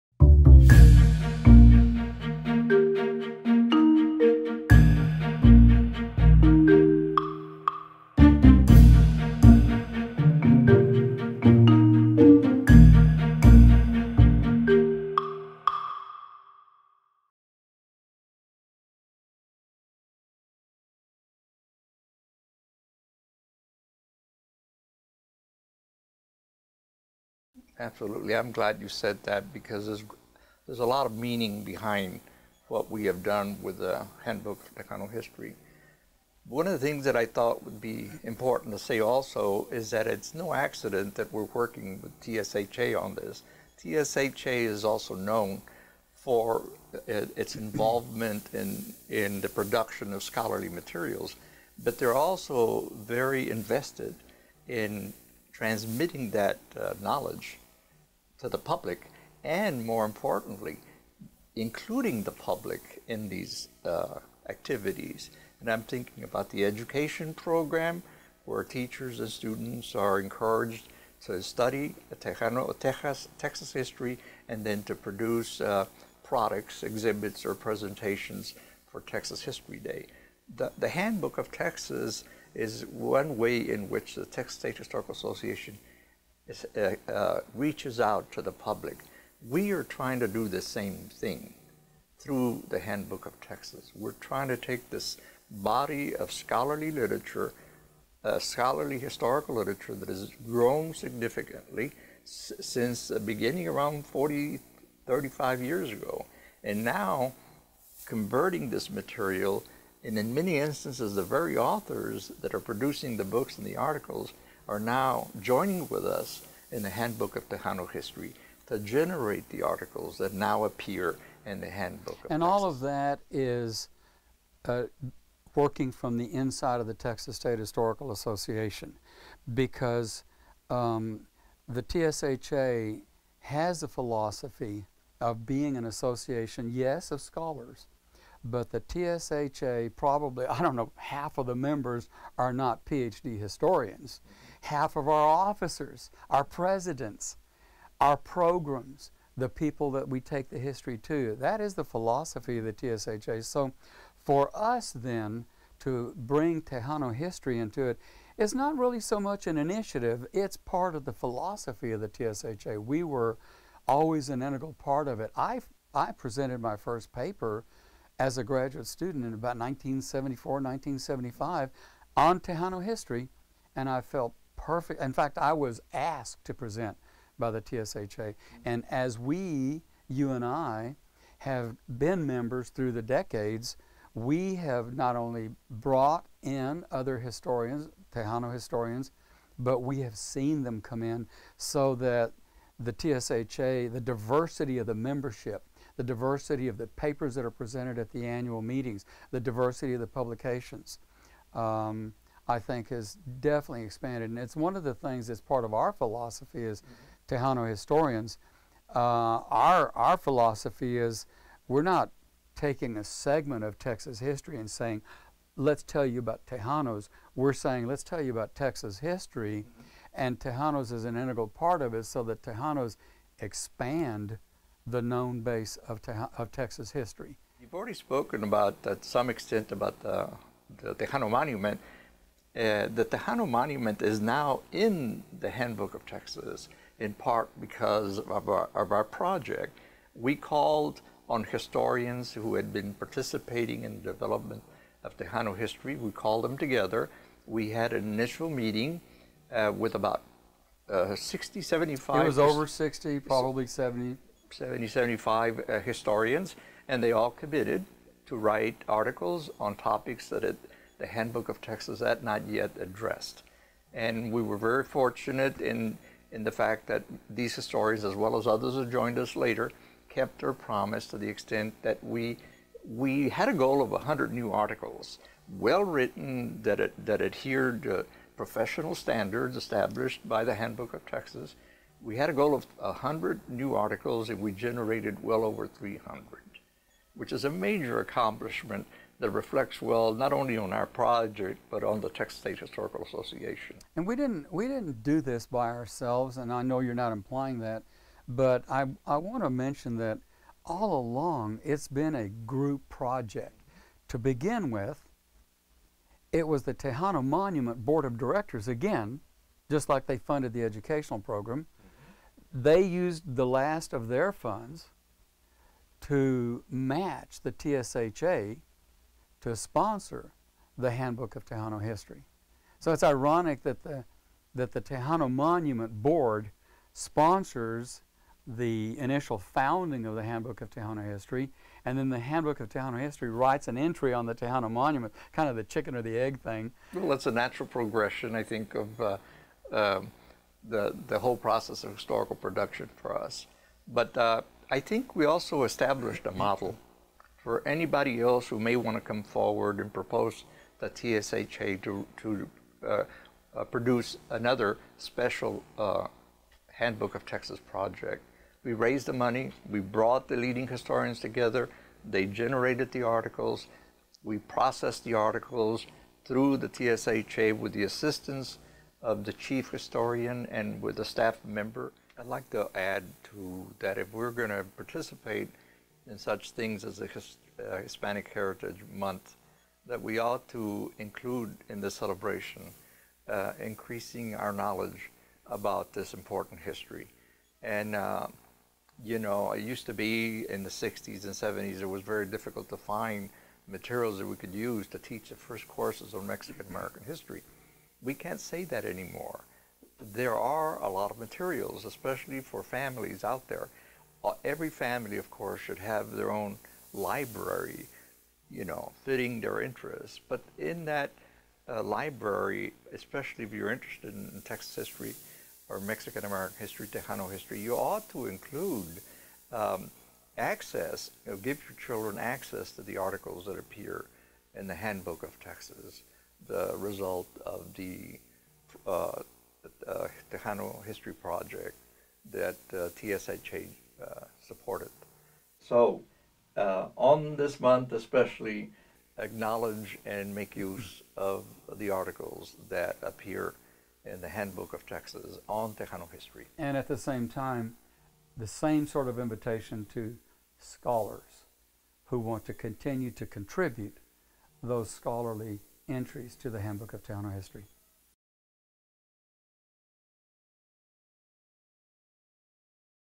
Texas Talks Clip - Talking Tejano History: TSHA Handbook of Tejano History
Dr. Emilio Zamora and Dr. Andres Tijerina discuss the background of the Handbook of Tejano History project and their work with the Texas State Historical Association.

From their full Texas Talks webinar "Talking Tejano History." Join TSHA to watch the full webinar